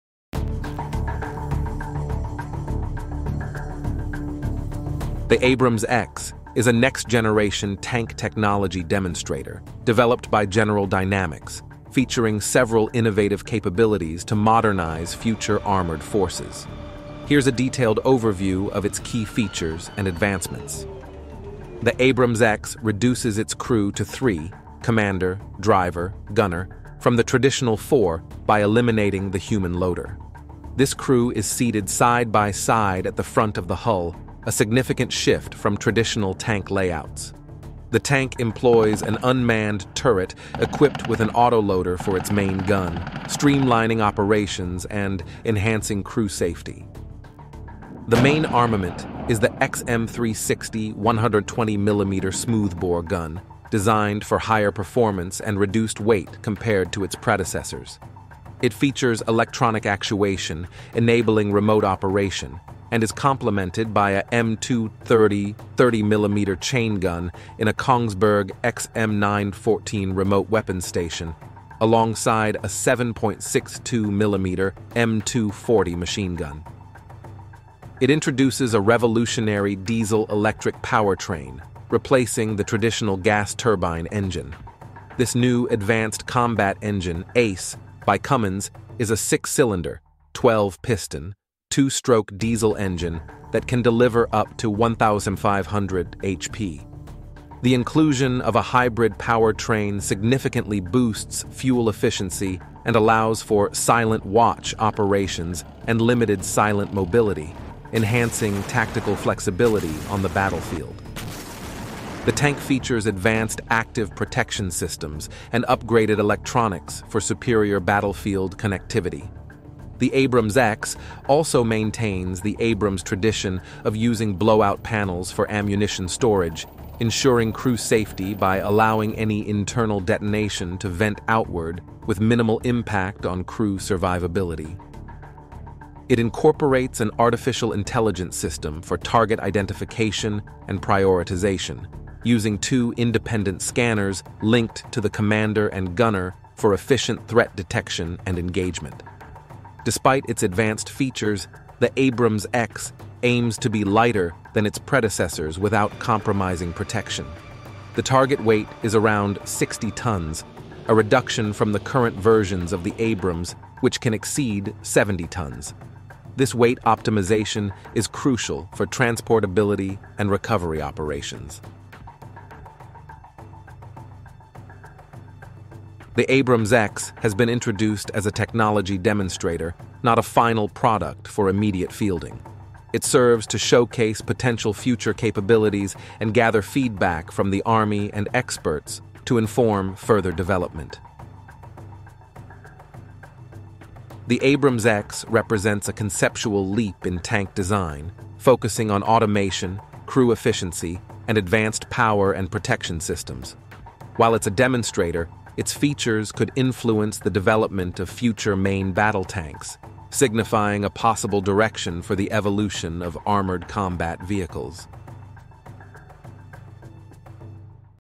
Introducing AbramsX: Revolutionizing the Battlefield
The AbramsX is a next-generation tank technology demonstrator developed by General Dynamics, featuring several innovative capabilities to modernize future armoured forces. Here's a detailed overview of its key features and advancements.